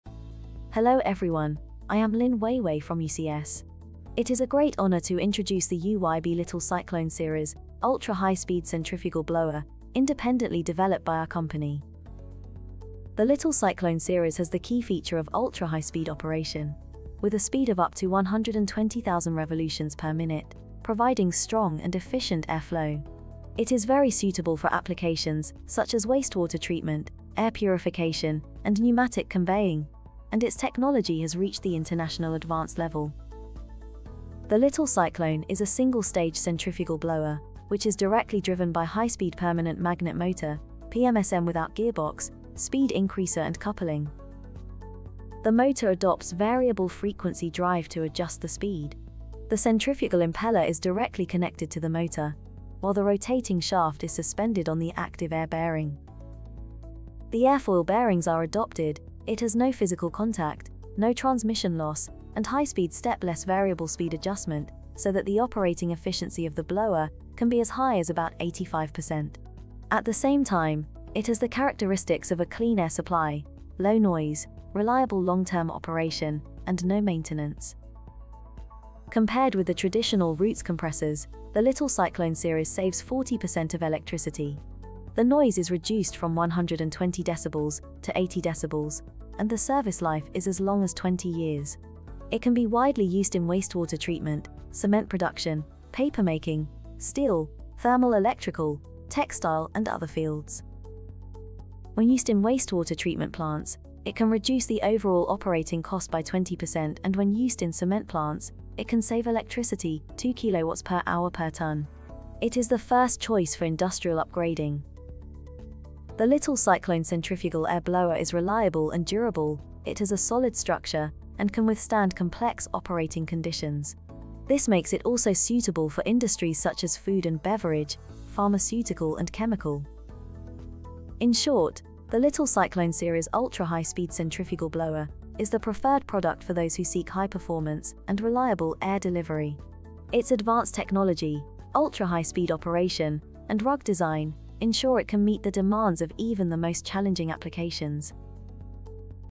UYB Little Cyclone Ultra-high-speed Centrifugal Air Blower with Contactless Bearing Technology
Introduction of the UYB Little Cyclone Series Ultra-high-speed Centrifugal Air blower independently developed by United Compressor Systems.

The Little Cyclone series has the key feature of ultra-high-speed operation, with a speed of up to 120,000 revolutions per minute, providing strong and efficient air flow. It is very suitable for applications such as wastewater treatment, air purification and pneumatic conveying, and its technology has reached the international advanced level.

The Little Cyclone is a single-stage centrifugal blower, which is directly driven by high-speed permanent magnet motor PMSM without gearbox, speed increaser and coupling

The motor adopts Variable Frequency Drive to adjust the speed. The centrifugal impeller is directly connected to the motor, while the rotating shaft is suspended on the active air bearing.

The airfoil bearings are adopted, it has no physical contact, no transmission loss, and high-speed step-less variable speed adjustment so that the operating efficiency of the blower can be as high as about 85%. At the same time, it has the characteristics of a clean air supply, low noise, reliable long-term operation and no maintenance.

Compared with the traditional Roots compressors, The Little Cyclone series saves 30%-40% of electricity, the noise is reduced from 120 decibels to 80 decibels, and the service life is as long as 20 years. It can be widely used in wastewater treatment, cement production, papermaking, steel, thermal Electrical, textile and other fields.

When used in wastewater treatment plants, it can reduce the overall operating cost by 20%, and when used in cement plants, it can save electricity 2kilo watts per hour per ton. It is the first choice for industrial upgrading.

The Little Cyclone centrifugal air blower is reliable and durable, it has a solid structure and can withstand complex operating conditions. This makes it also suitable for industries such as food and beverage, pharmaceutical and chemical.

In short, the Little Cyclone series ultra-high-speed centrifugal blower is the preferred product for those who seek high performance and reliable air delivery. Its advanced technology, ultra-high-speed operation and rugged design ensure it can meet the demands of even the most challenging applications.